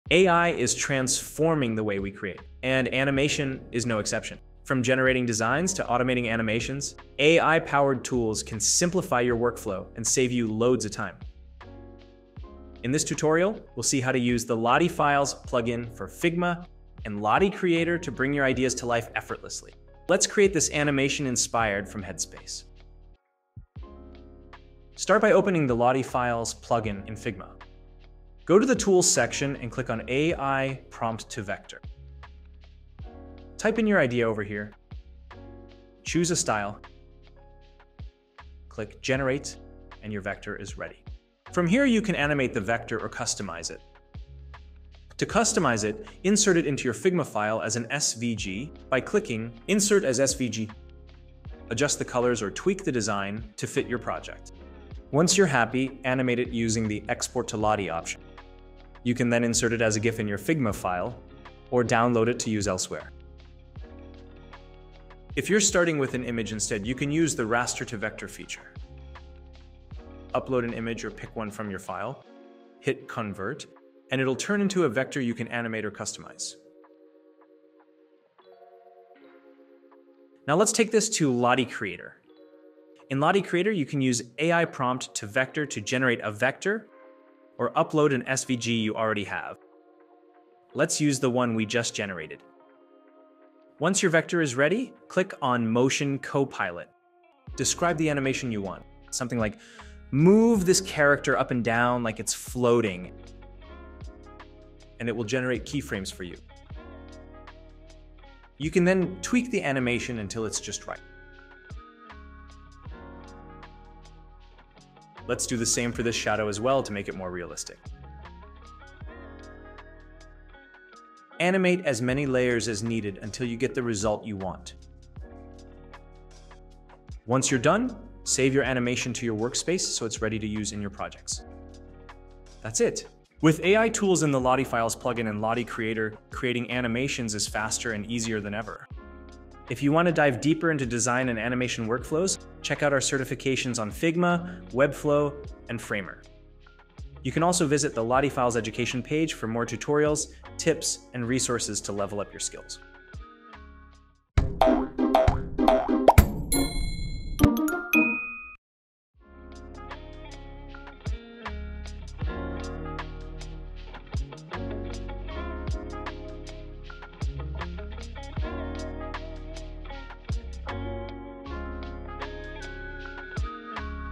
Mastering AI Tools in LottieFiles: A Guide for Figma and Lottie Creator Users
Learn how to use AI-powered features in tools such as LottieFiles for Figma and Lottie Creator to transform your ideas into motion magic in no time.

Timestamps:

00:00 Introduction to AI in Animation
00:25 AI Prompt to Vector in Figma
00:43 Customizing the vector
00:55 Animating with Export to Lottie option
01:05 Using Raster to Vector
01:21 AI Prompt to Vector in Lottie Creator
01:36 Motion Copilot